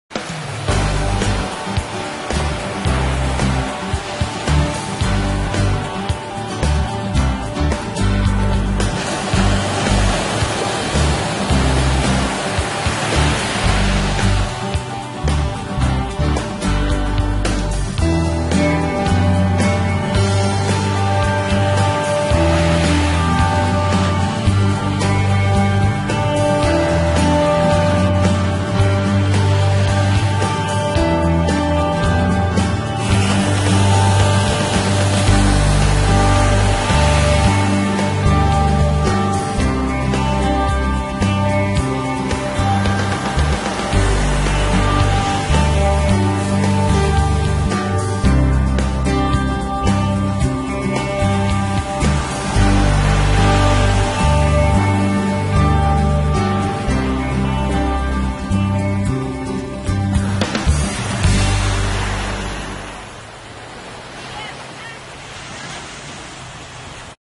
Wabasso Beach view from the boardwalk
Great view of Wabasso Beach (from the boardwalk) on the Treasure Coast of Florida. for more great info.